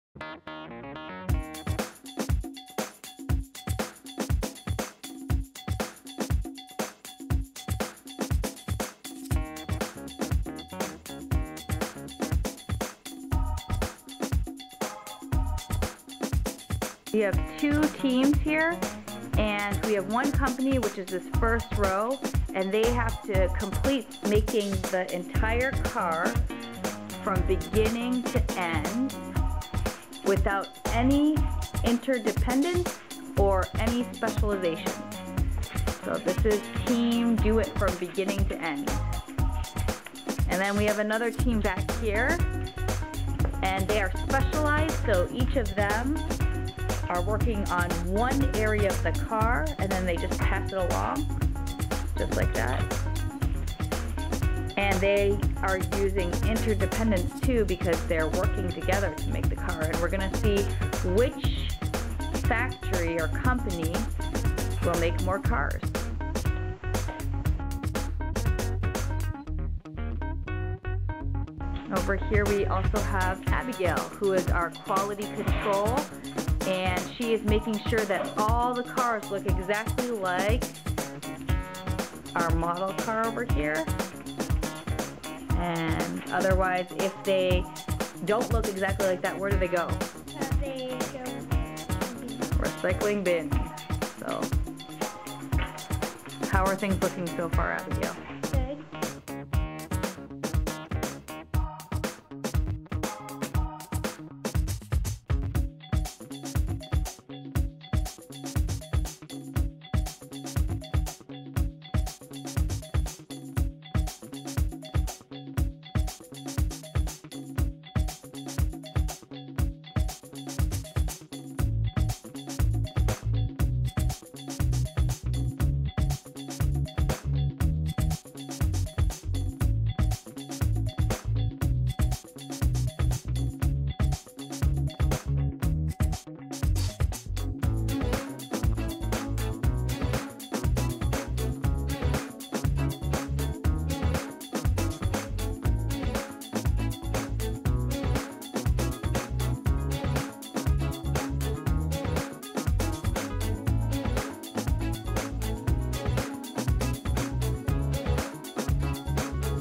Interdependence and Specializations Movie
This is a video of the 3rd graders doing an activity that helped them to understand why producers need to use interdependence and specializations as they produce goods for consumers.